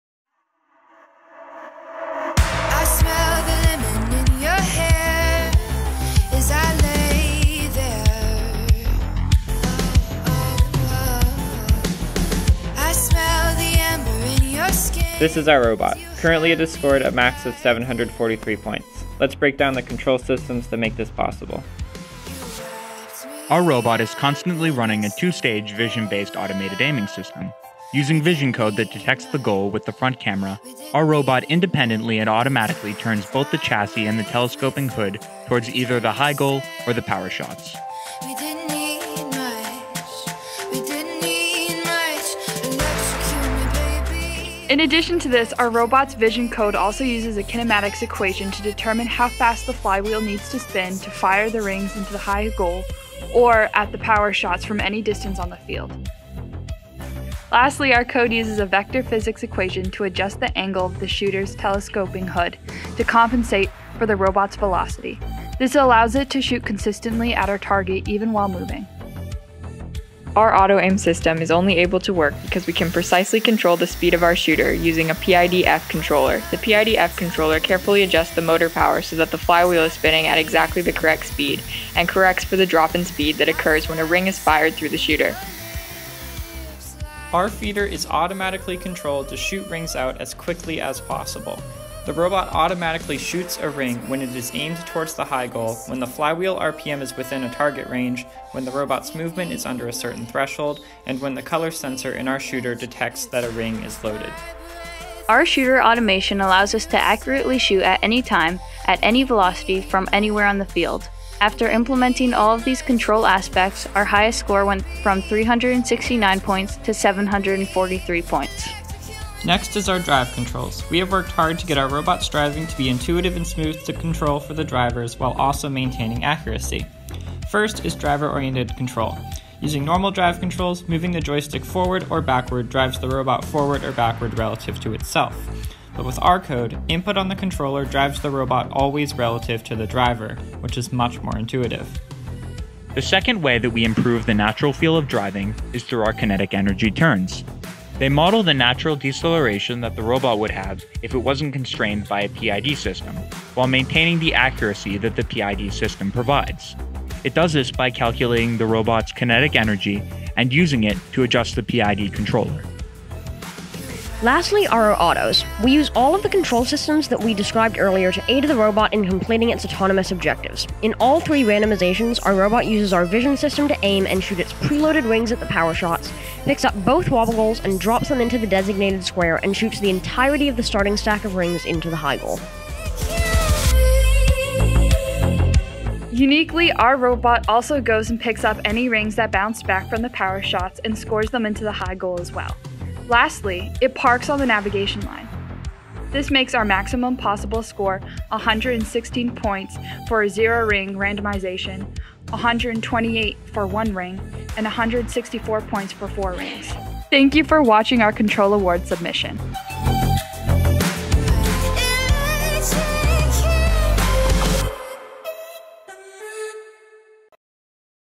MA Control Award Winner | Wolfpack Machina 18438
Winning control award video submission for the remote MA FTC state tournament in May

Our second channel (more videos of our robots): youtubetoo.brickwolves.com